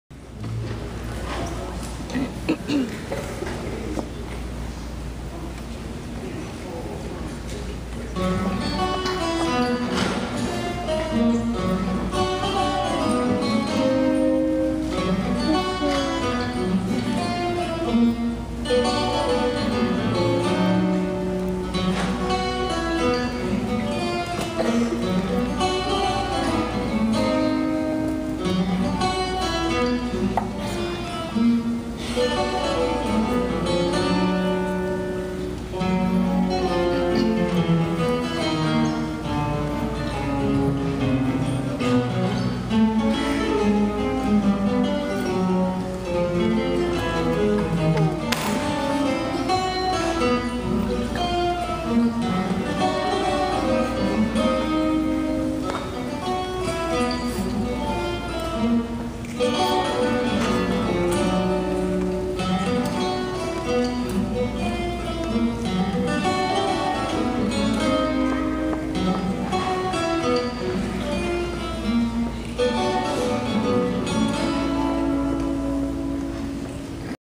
Guitar concert RCHS
high school spring concert at Royse City